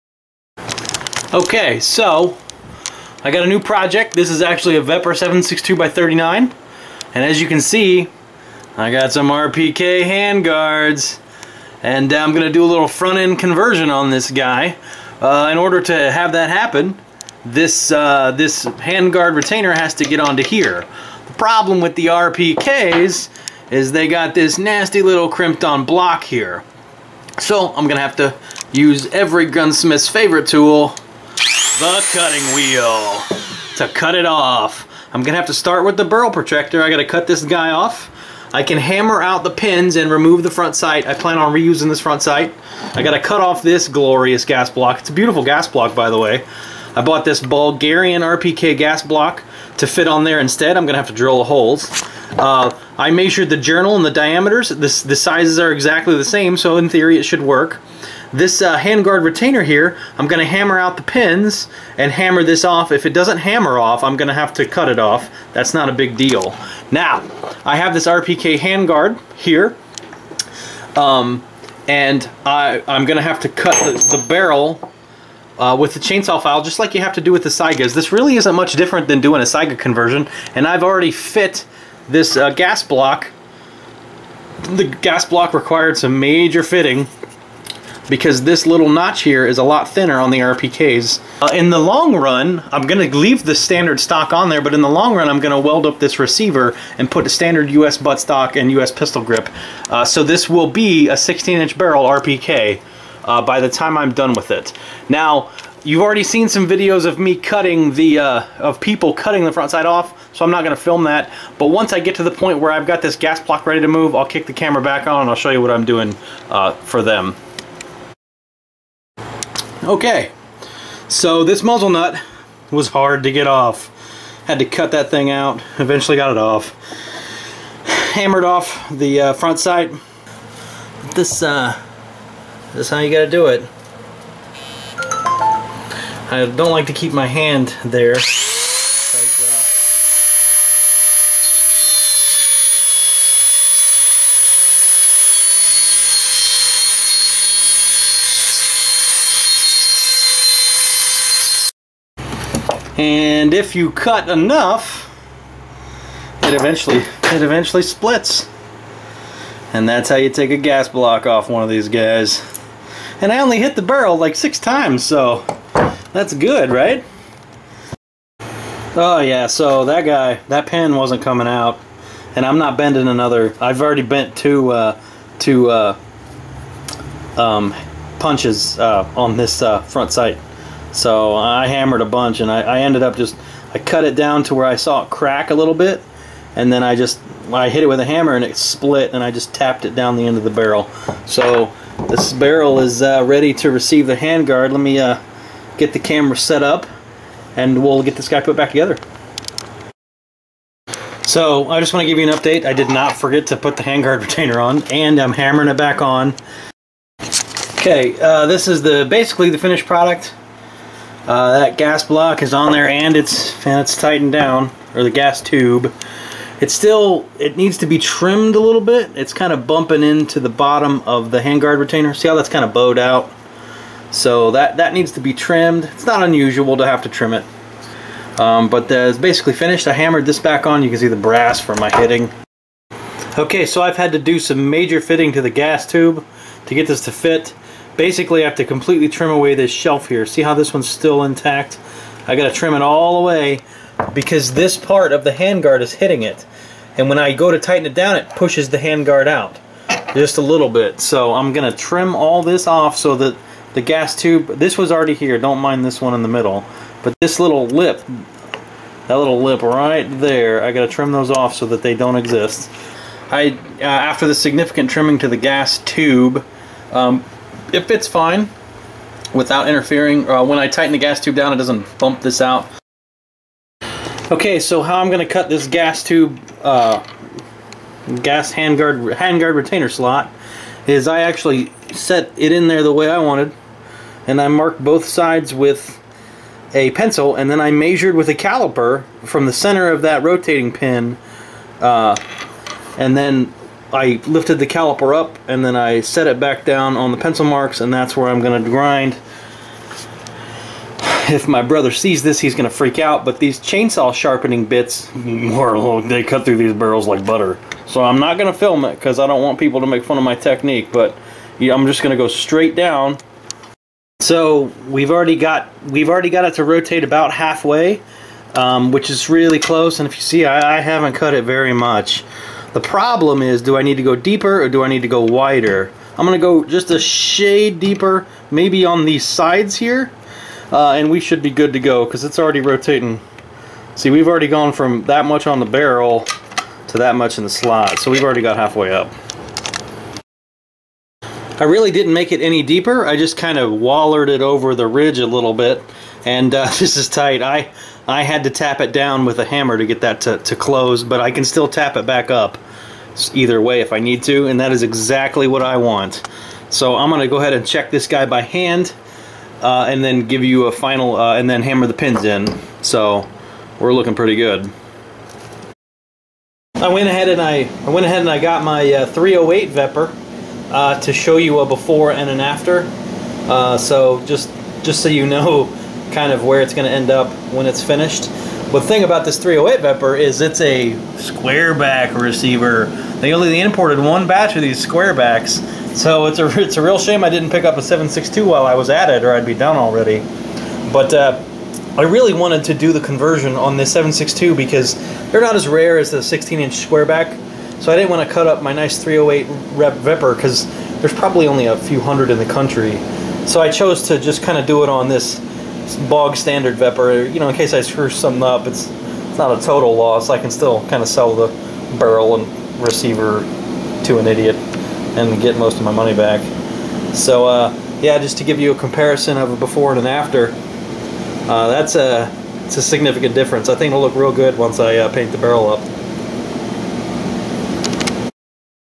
Russian Vepr RPK Rifle Build Conversion
Here we take a standard Vepr and convert the barrel to be like a real RPK!!